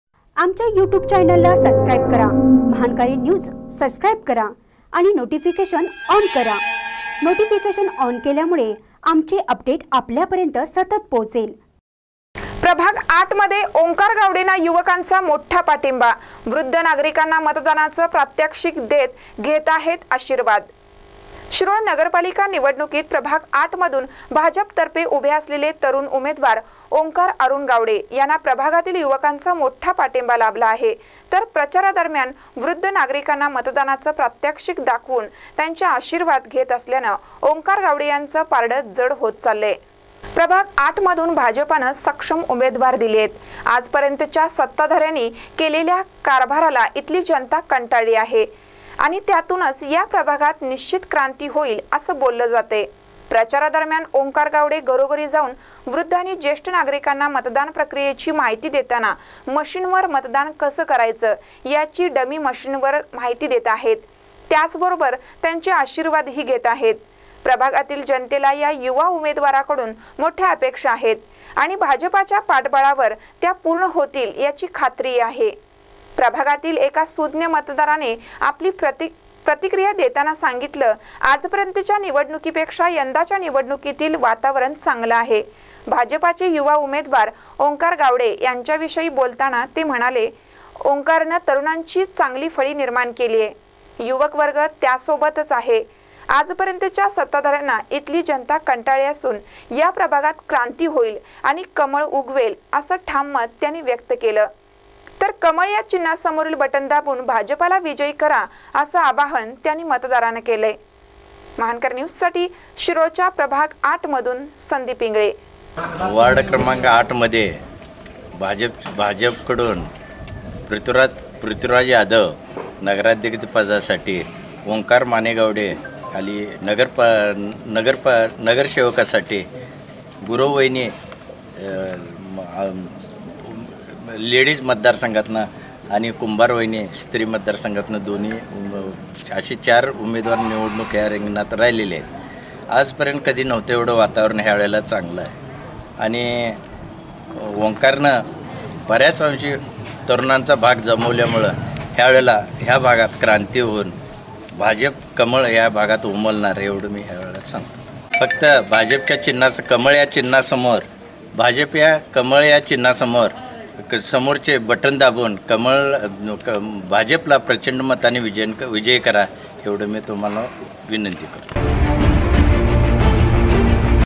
प्रभाग ८ मध्ये ओंकार गावडेंना युवकांचा मोठा पाठींबा : वृध्द नागरिकांना
प्रभाग ८ मध्ये ओंकार गावडेंना युवकांचा मोठा पाठींबा : वृध्द नागरिकांना मतदानाचं प्रात्यक्षिक देत घेताहेत आशिर्वाद